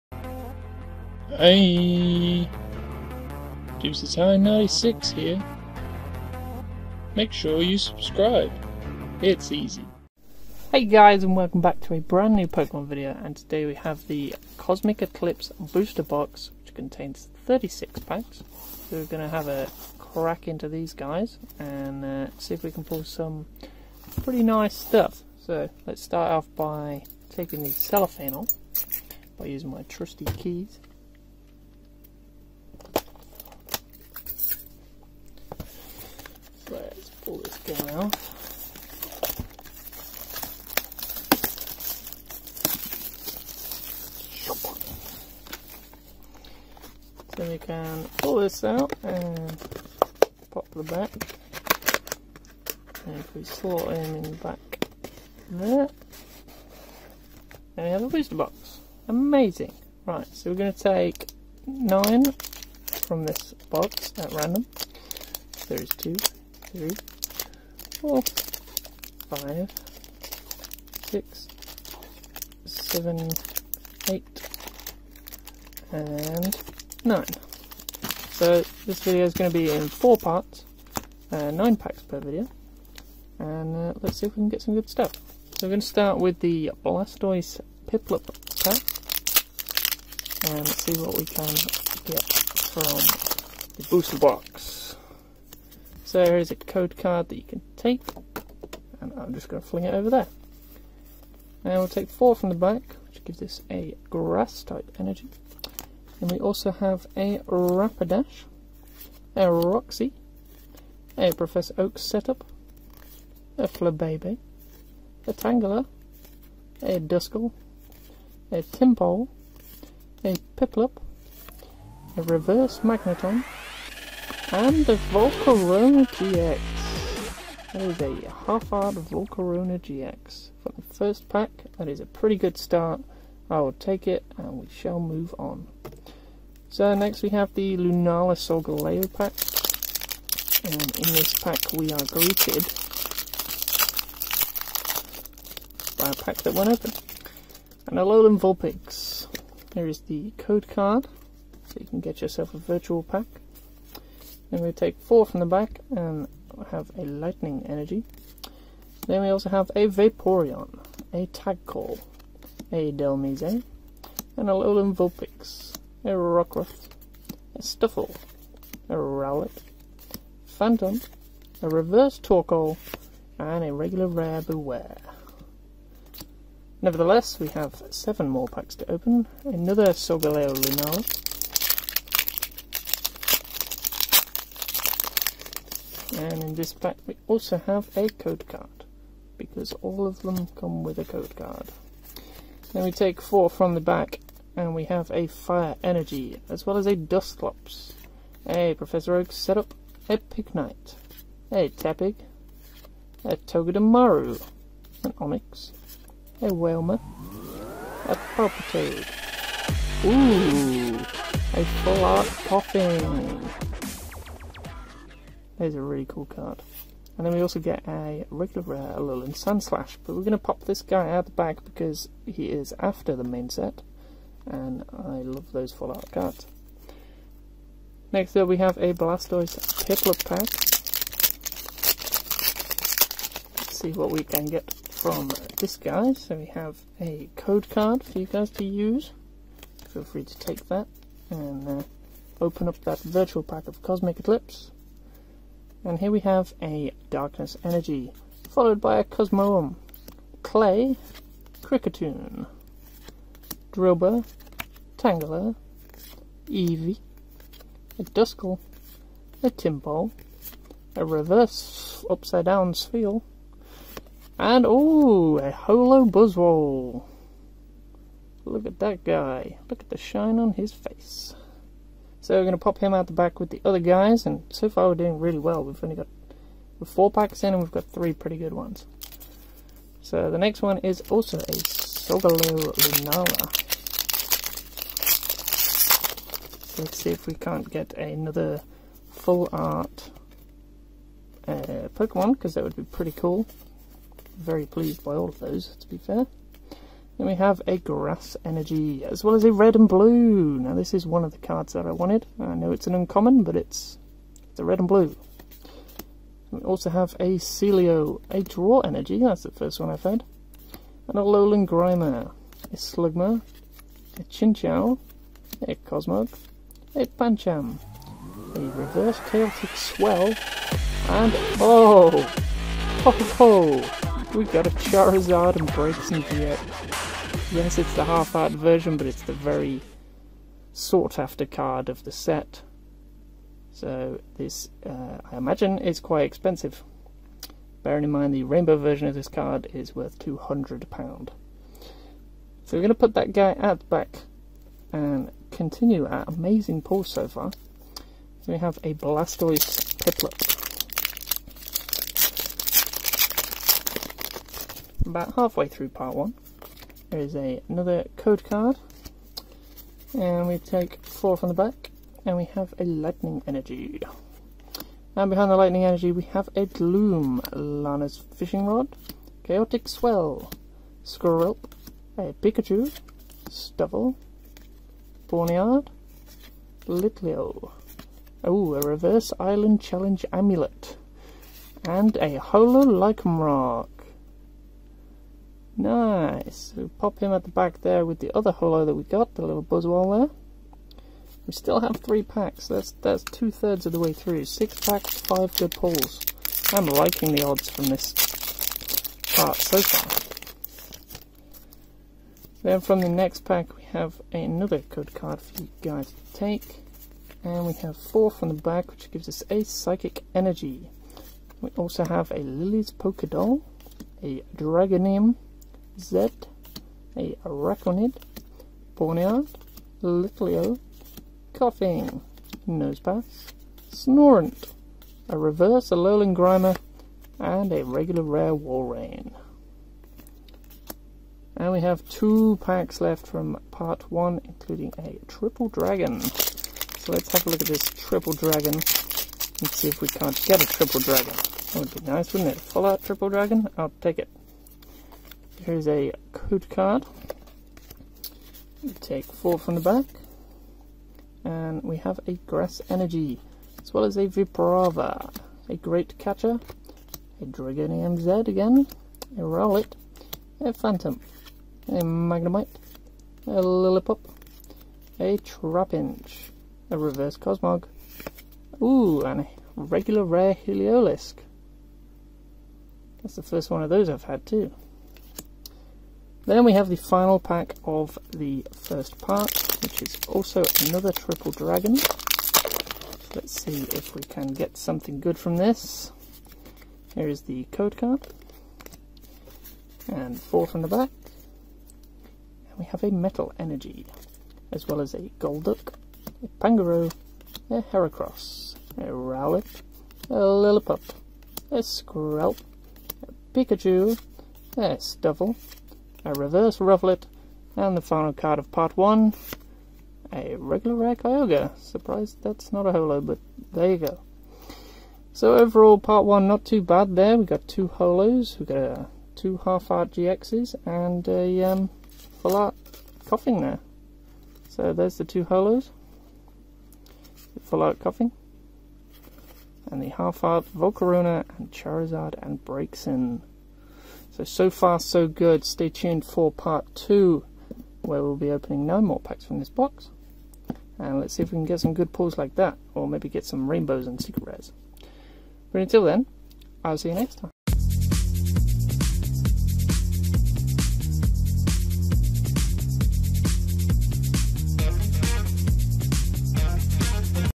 Pokemon | Cosmic Eclipse | Booster Box Opening | Part 1
Here I open the Brand New Booster Box containing 36 Packs of Sun and Moon: Cosmic Eclipse!
Check out the pulls in Part 1!